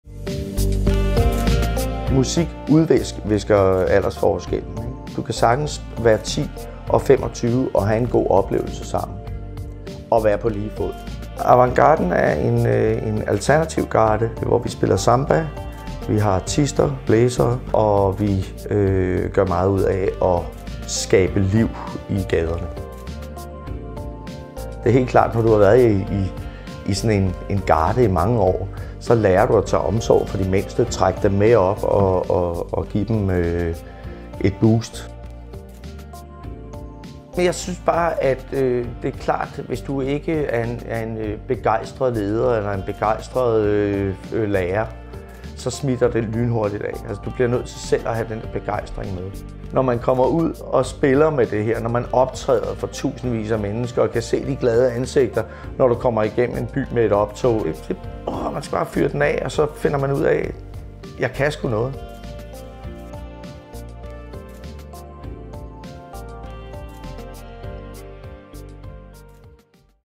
Næstved har talent - Næstved Avantgarde
Uden miljøer, hvor talenter kan udforske og udvikle deres talenter, ingen talenter. Så simpelt er det.
Her får I video #8, hvor Jens Ulrik Simonsen fortæller hvordan han og de andre musikundervisere hjælper talenter på vej i Næstved Avantgarde med lige dele fællesskab, læring og sammenspil. Og så har de det det hammersjovt! Mød dem på gader og stræder til alle slags arrangementer - du kan kende dem på det høje humør og de fede outfits!